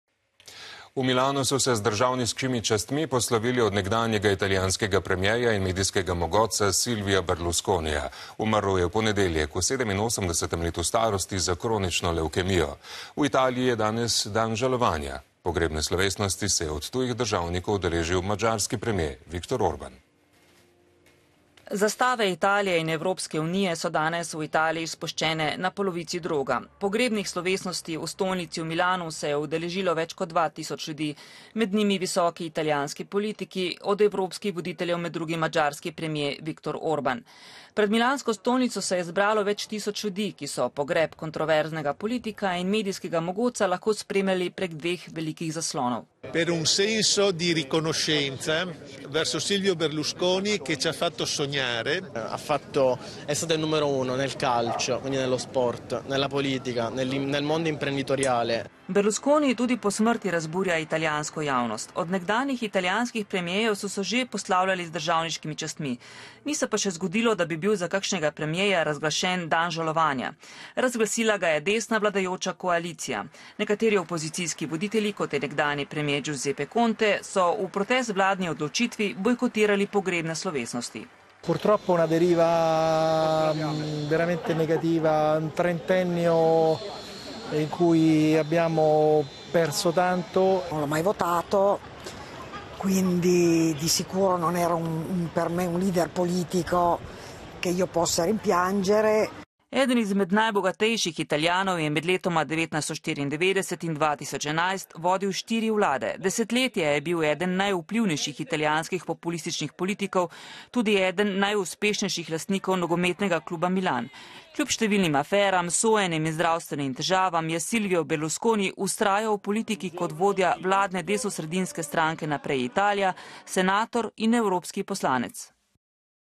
Pogreb Berlusconija z državniškimi častmi - TV Slovenia 14.6.2023
Pred stolnico se je zbralo približno 15.000 ljudi, pogrebne slovesnosti pa se jih je udeležilo približno 2000, ob svojcih med drugim tudi italijanski predsednik Sergio Mattarella in premierka Giorgia Meloni s številnimi člani vlade.
"Silvio Berlusconi je bil zagotovo človek politike. Toda kaj lahko v tem času, ko se poslavljamo od Berlusconija in molimo, rečemo o njem? Bil je človek, poln življenja, ljubezni in veselja," je na pogrebu dejal milanski nadškof Mario Delpini.
Ob krsti so stali njegovi otroci Marina, Pier Silvio, Barbara, Eleonora in Luigi ter njegova partnerica Marta Fascina.
Pogreba naj bi se udeležili tudi politiki iz tujine, med njimi madžarski premier Viktor Orban in katarski emir Tamim bin Hamad Al Thani. Ni pa bilo vodje opozicijskega Gibanja petih zvezd in nekdanjega premierja Giuseppeja Conteja, Nicole Fratoiannija iz stranke Italijanska levica ter vodje Zelenih Angela Bonellija, poroča italijanska tiskovna agencija Ansa.
Na trgu pred gotsko katedralo so postavili dva velika zaslona, ki sta omogočila, da so ljudje lahko spremljali pogrebno slovesnost. Več kot milijon ljudi pa si je slovesnost ogledalo na televiziji.
Berlusconi, ki je bil eden najbogatejših Italijanov, je bil desetletja v vrhu italijanske politike. Kljub številnim korupcijskim in drugim aferam ter sojenjem pa tudi zdravstvenim težavam je do konca ostal v politiki kot vodja vladne desnosredinske stranke Naprej, Italija, senator in evropski poslanec. Med letoma 1994 in 2011 je vodil štiri vlade.
Na javnih poslopjih po državi in na italijanskih diplomatskih predstavništvih v tujini so v znak žalovanja italijanske zastave in zastave Evropske unije izobešene na pol droga.